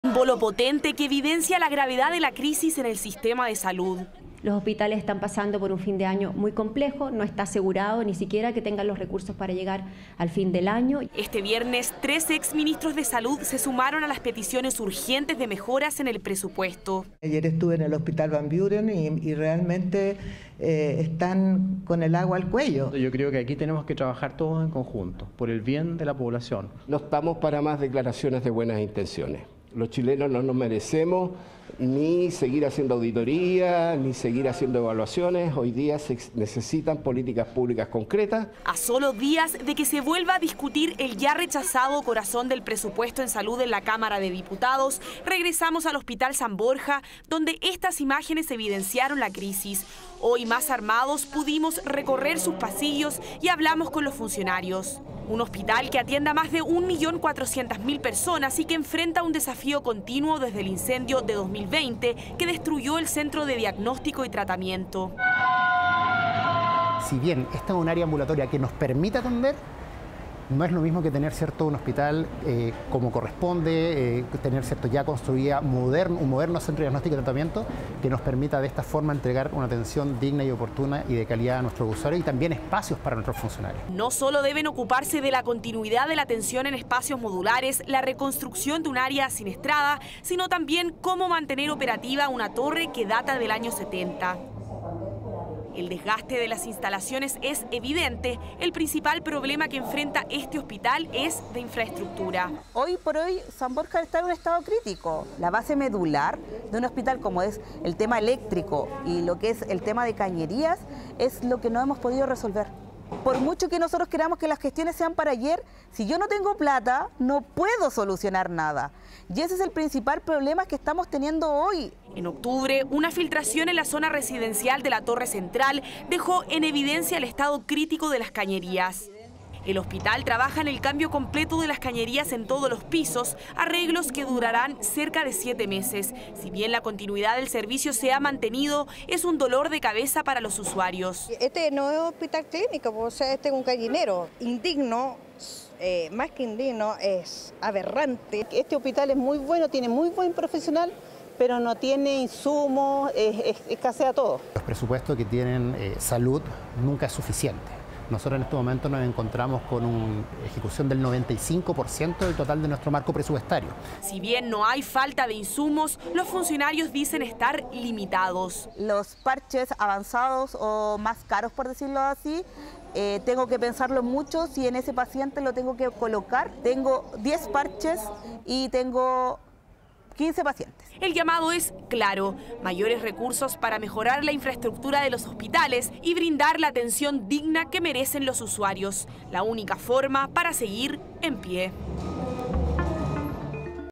Hospital San Boja: cañerías viejas y un hospital en ruinas evidencian las crisis | 24 Horas
Este jueves se volvió a rechazar el corazón de presupuesto en salud..Volvimos al Hospital San Borja y presenciamos los problemas de infraestructura, como cañerías que ya cumplieron su vida útil y un centro que necesita ser reconstruido. Por eso, ex ministros de salud se reunieron con el Colegio Médico para pedir mayor urgencia en la discusión del presupuesto.